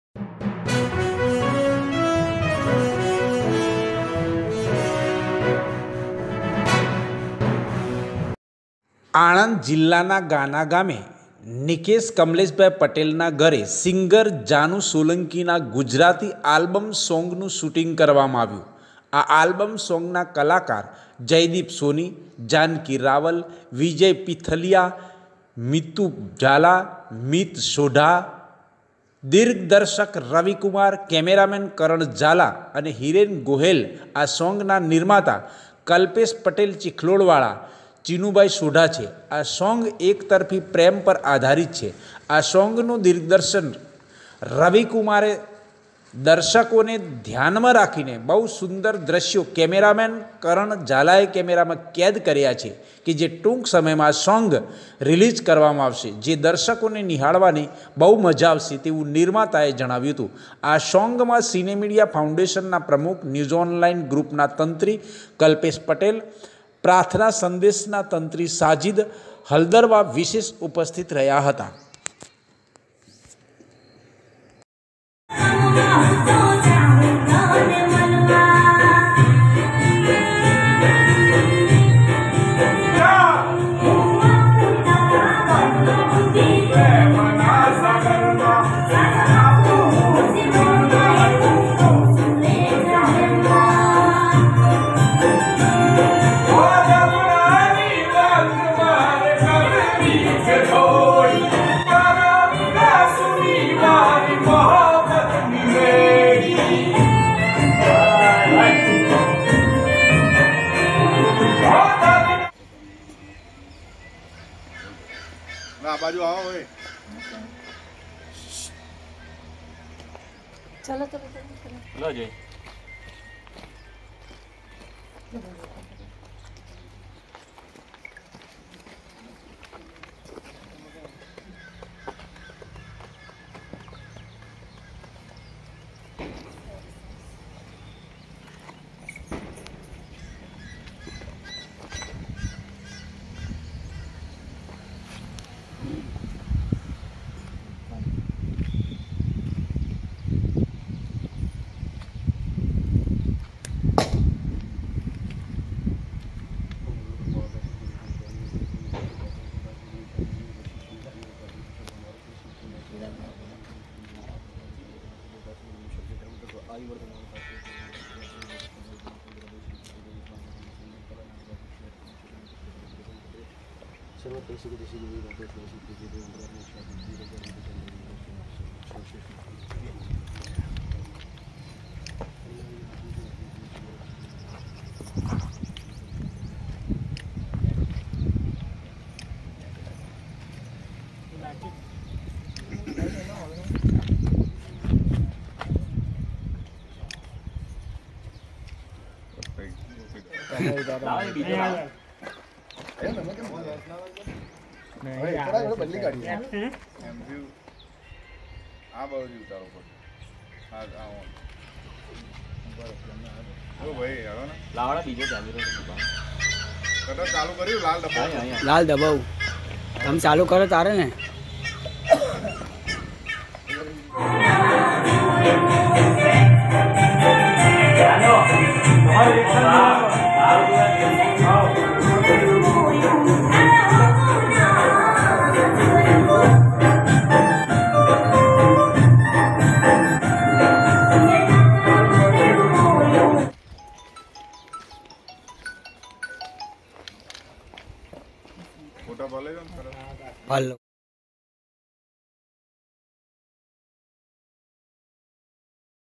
આણંદ જીલાના ગાના ગામે નિકેશ કમલેશભાઈ પટેલના ઘરે સિંગર જાનું સોલંકીના ગુજરાતી આલ્બમ સોન્ગનું શૂટિંગ
𝗟𝗮𝘁𝗲𝘀𝘁 𝗡𝗲𝘄𝘀